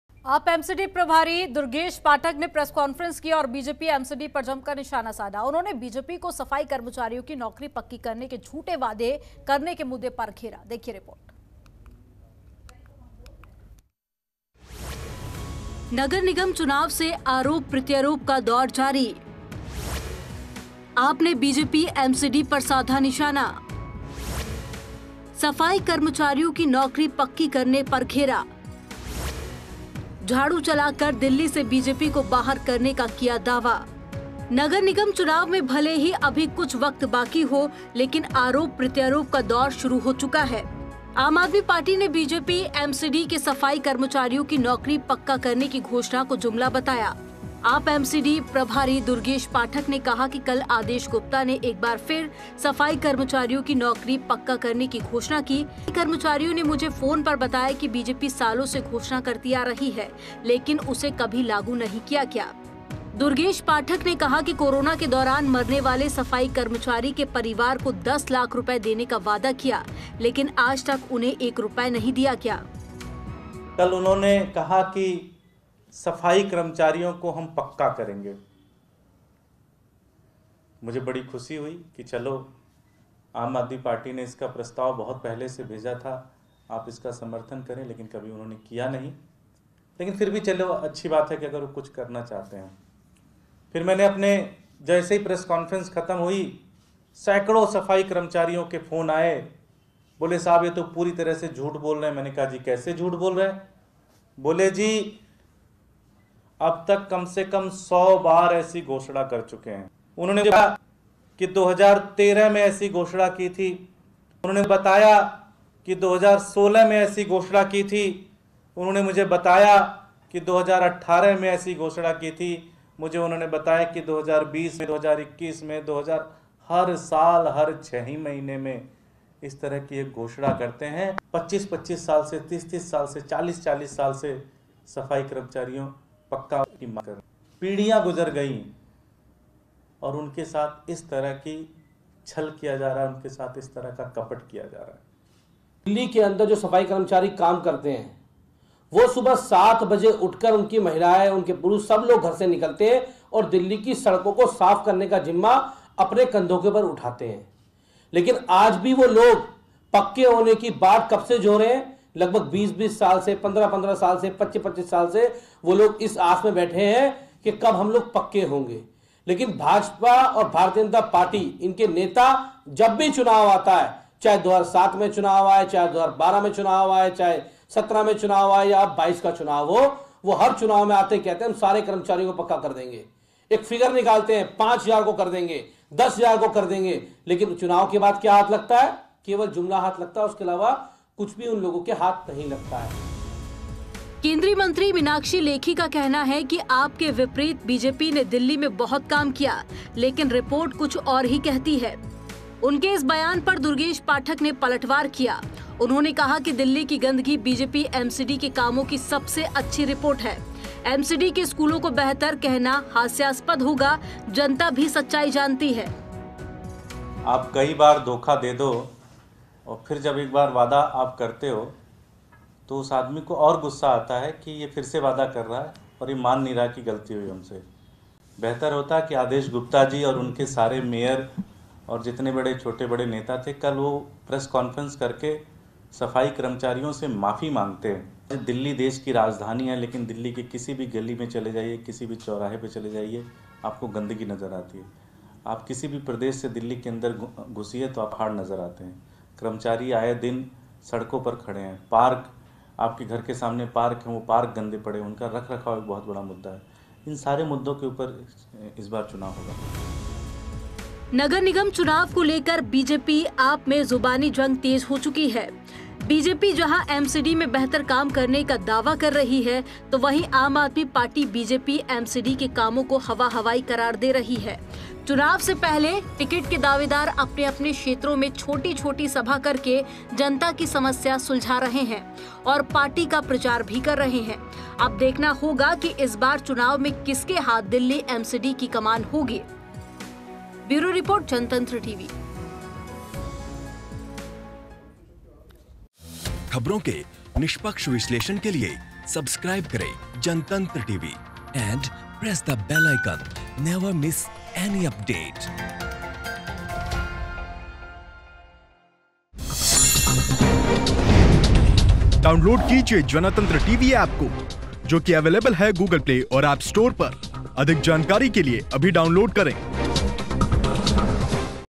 Durgesh Pathak Allegations On BJP | AAP Will March Against MCD | MCD Delhi News
In This Video:
Durgesh Pathak Allegations On BJP,
AAP Will March Against MCD,
MCD Delhi News,